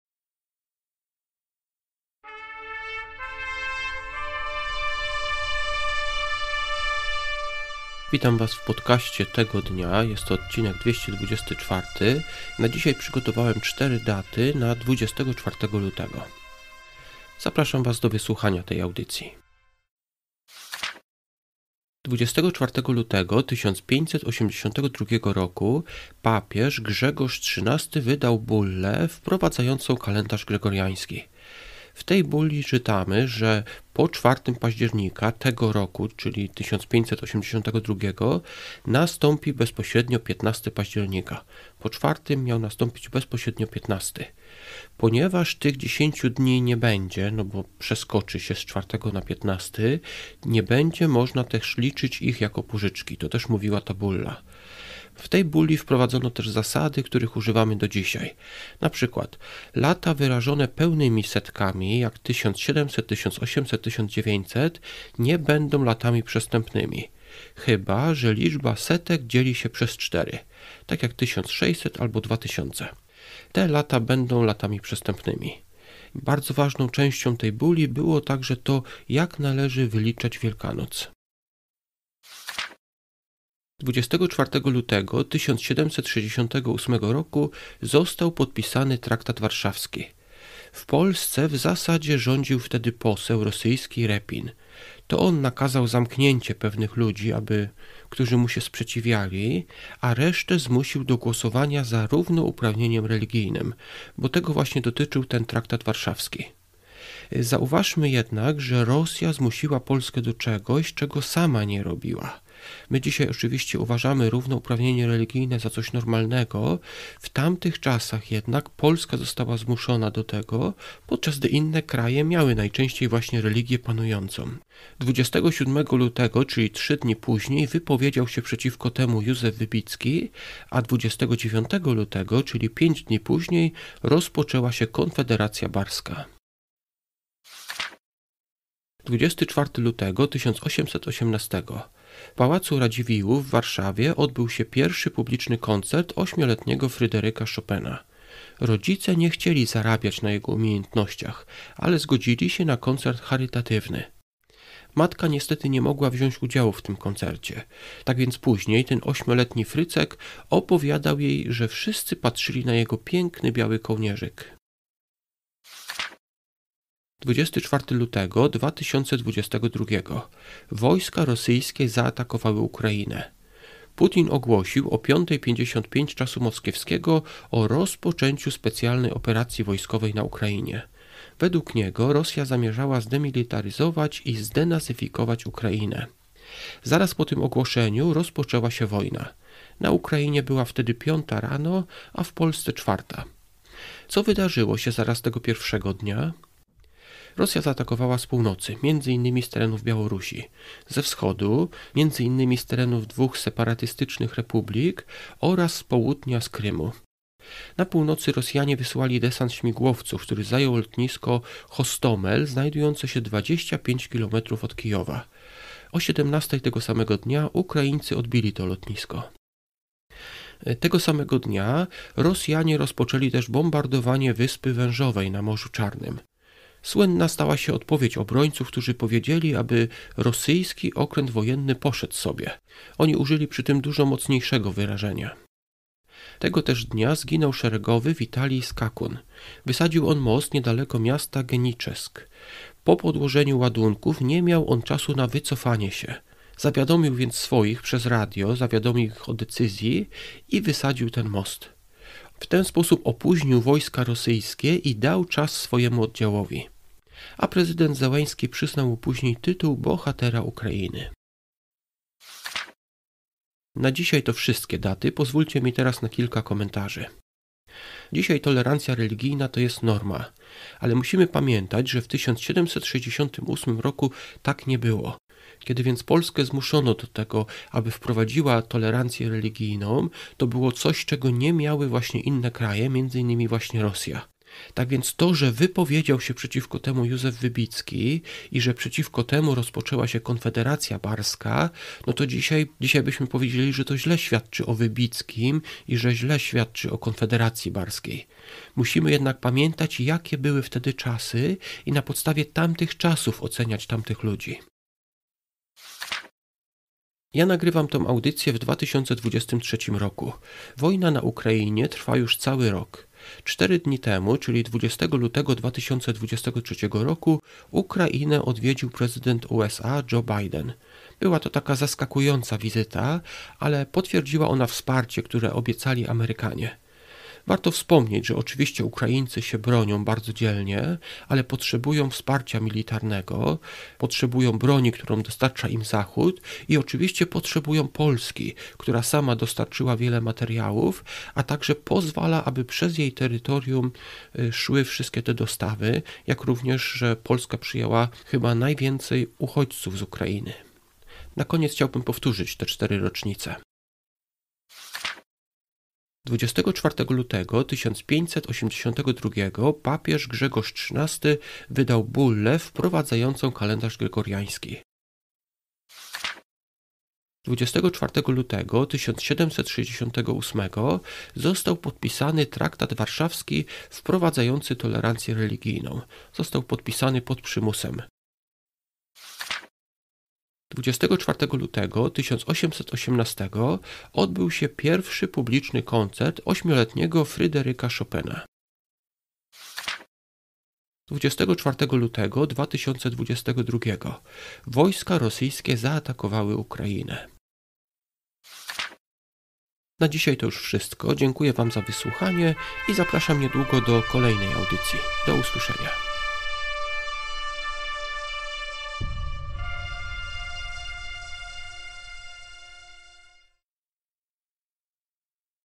Rocznice: 24 lutego
1582 – Papież Grzegorz XIII wydał bullę wprowadzającą kalendarz gregoriański.
1768 – Został podpisany traktat warszawski wprowadzający tolerancję religijną.
1818 – Odbył się pierwszy publiczny koncert 8-letniego Fryderyka Chopina.
2022 – Wojska rosyjskie zaatakowały Ukrainę.